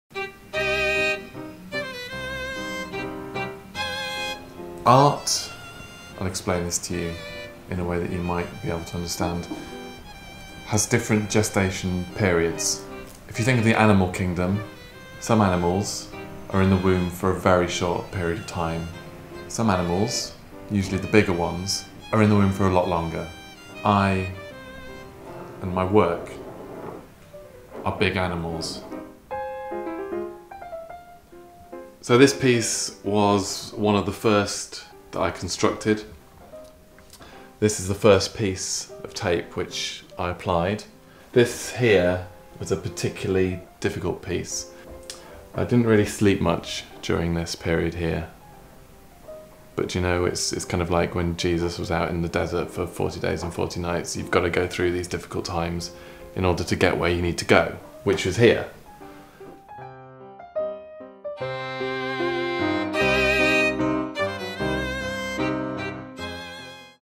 CONCEPTUAL ART EXPLAINED BY ARTIST TOBY GLASS
Artist Toby Glass explains conceptual art to the ignorant public.
New comedy films every week!